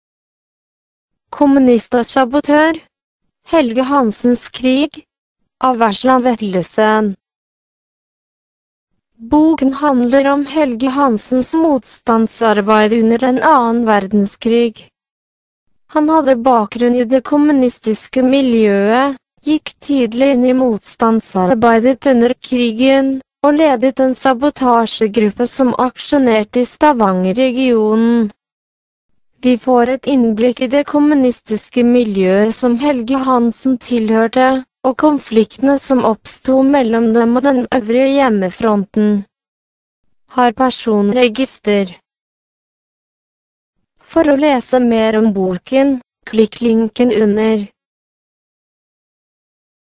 Kommunist og sabotør - Vesla Vetlesen
Kommunist og sabotør : Helge Hansens krig. Av Vesla Vetlesen. Boken handler om Helge Hansens motstandsarbeid under d...